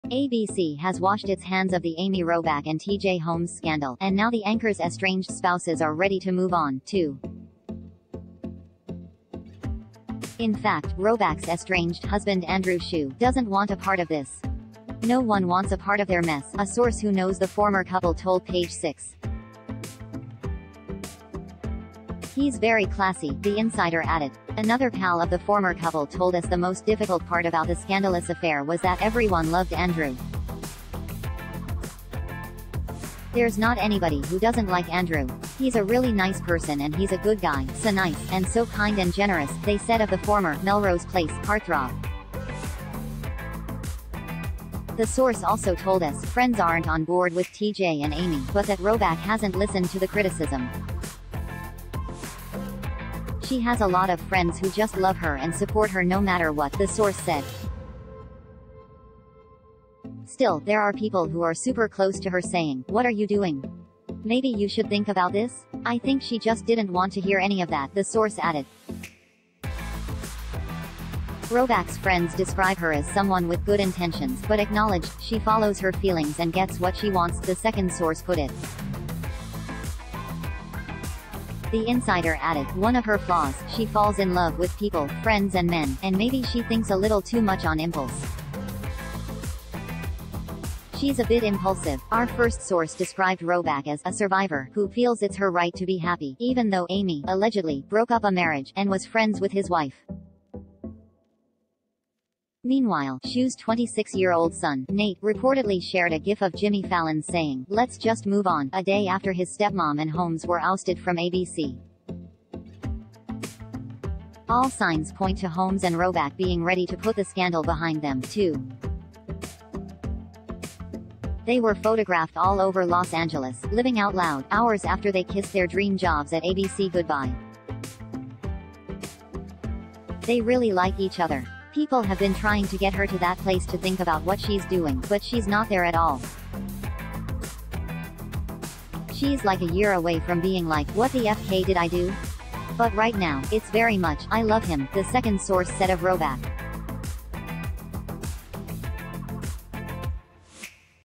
Andrew Shue‘too classy’ for messy Amy Robach and T.J. Holmes saga
ABC has washed its hands of the Amy Robach and T.J. Holmes scandal — and now the anchors’ estranged spouses are ready to move on, too.    In fact, Robach’s estranged husband Andrew Shue “doesn’t want a part of this. No one wants a part of their mess,” a source who knows the former couple told Page Six.    “He’s very classy,” the insider added.    Another pal of the former couple told us the most difficult part about the scandalous affair was that “everyone loved Andrew.”   “There’s not anybody who doesn’t like Andrew. He’s a really nice person and he’s a good guy — so nice, and so kind and generous,” they said of the former “Melrose Place” heartthrob.    The source also told us “friends aren’t on board with T.J. and Amy,” but that Robach hasn’t listened to the criticism.    “She has a lot of friends who just love her and support her no matter what,” the source said.    Still, “there are people who are super close to her saying, ‘What are you doing? Maybe you should think about this?’ I think she just didn’t want to hear any of that,” the source added.    Robach’s friends describe her as someone with good intentions, but acknowledged, “she follows her feelings and gets what she wants,” the second source put it. The insider added, “One of her flaws: She falls in love with people — friends and men — and maybe she thinks a little too much on impulse. She’s a bit impulsive.”    Our first source described Robach as “a survivor” who “feels it’s her right to be happy,” even though “Amy [allegedly] broke up a marriage, and was friends with his wife.”   Meanwhile, Shue’s 26-year-old son, Nate, reportedly shared a GIF of Jimmy Fallon saying, “Let’s just move on,” a day after his stepmom and Holmes were ousted from ABC.    All signs point to Holmes and Robach being ready to put the scandal behind them, too. They were photographed all over Los Angeles, living out loud, hours after they kissed their dream jobs at ABC goodbye.    “They really like each other. People have been trying to get her to that place to think about what she’s doing, but she’s not there at all. She’s like a year away from being like, ‘What the f–k did I do?’ But right now, it’s very much, ‘I love him,'” the second source said of Robach.